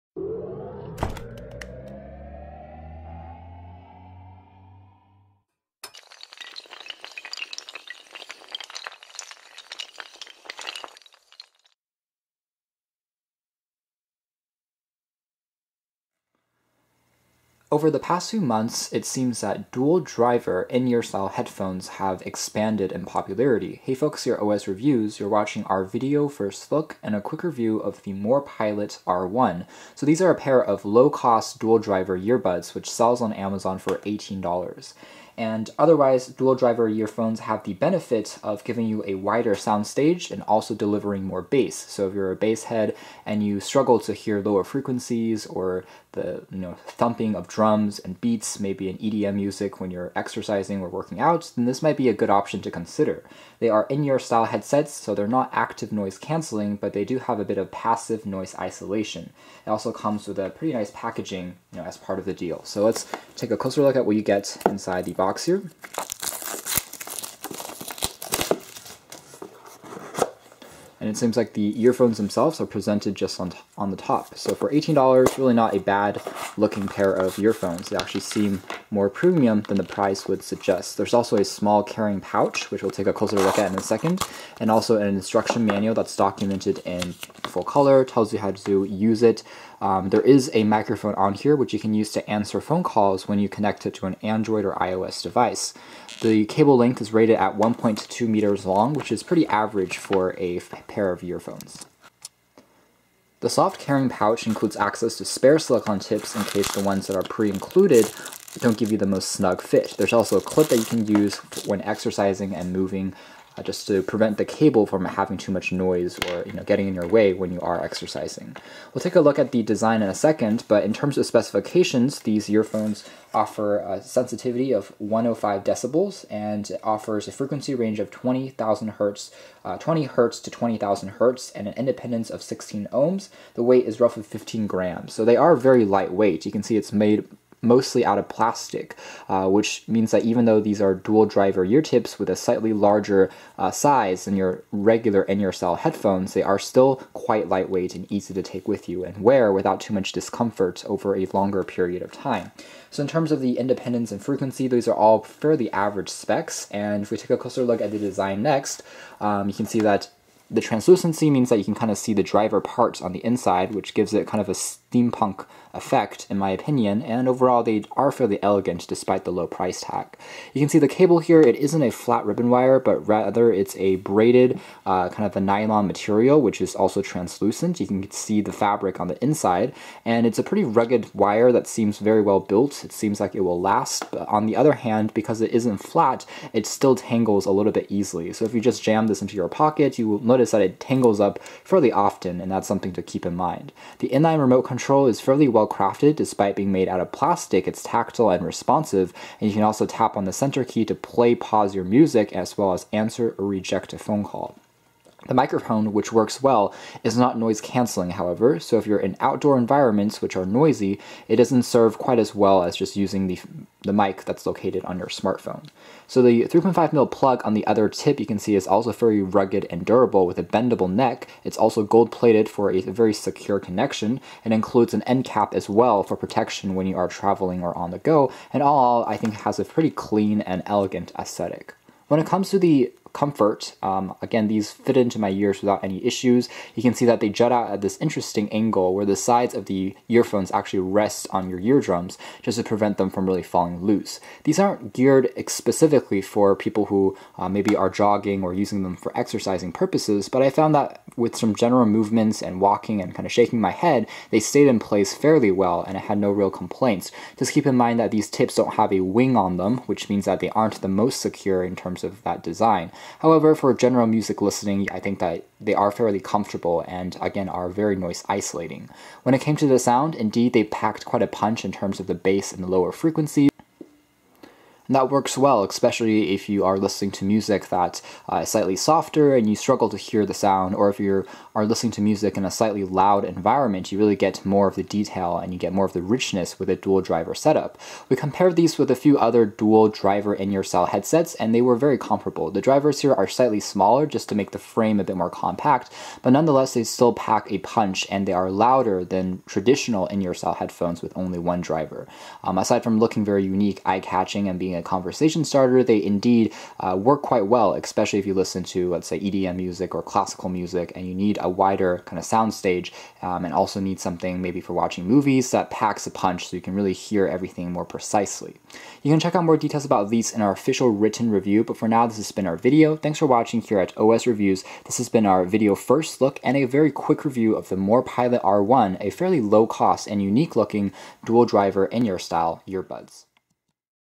REVIEW: Morpilot R1 Dual Driver - In-Ear Headphones
Morpilot Dual Dynamic Driver Wired Earbuds

Details:
Dual Drivers - deliver clean, natural, and well-balanced sound, The bass is full, the mids are articulate, the highs are sparkling, and the soundstage is wide.
Ultimate Comfort-Ergonomic design with 6 pairs of silicone ear tips,perfectly conform to the shape of your ears and freeing you to enjoy your music.
Made for Music- higher sensitivity and wider frequency response ensure a better listening experience. Suitable for all genres of music (Pop, Rock, Country, Hip-Hop, Jazz)
Universal Compatibility - With 1.2m sturdy non-kinking cord & 3.5mm gilded plug, these headphones are compatible with all 3.5mm jack devices including iPhone 6s, 6s Plus, 6, and 5S, iPad, Samsung Galaxy and Note Series, Nexus, HTC, Laptops, Tablets, etc.

The Morpilot earphone is equipped with two drivers and seamless ergonomic fit,by separating the high-end frequencies from the mids and the bass your music comes alive with exquisite detail and emotional power.

Specification:
♫ Sensitivity: 105 dB ± 3dB at 1K Hz
♫ Max. input power: 3mW
♫ Frequency range: 20 Hz~20K Hz
♫ Impedance: 16 Ohm±15% at 1K Hz
♫ Weight: 15g/0.53oz

In the Box:
1x Morpilot in-ear earphone
6x Pairs Multi-sized eartips
1x Pocket-sized soft carrying case
2x Shirt Clip
1x Quick Guide